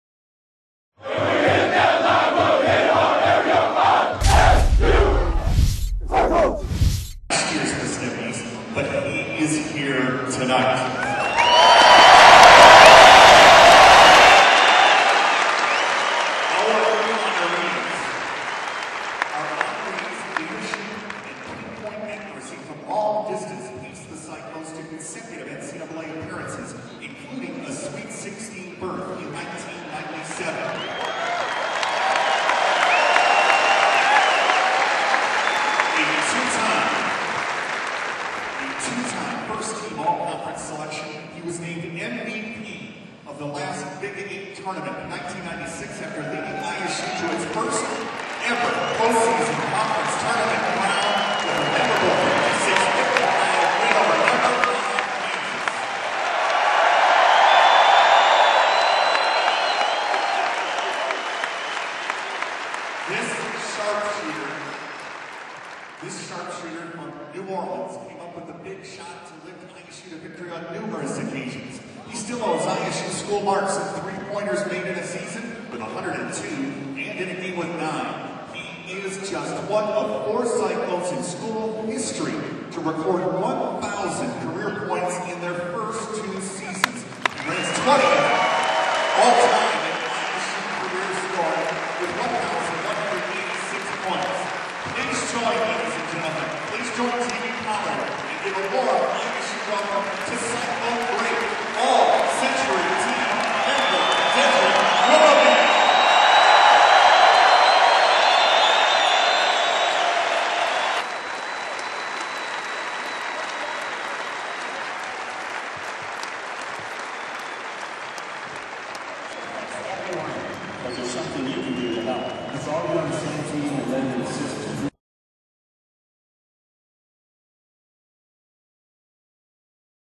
Willoughby ISU All-Century Team halftime presentation
Dedric Willoughby was named to Iowa States All-Century team for men's basketball in celebration of  100 years of mens' hoops.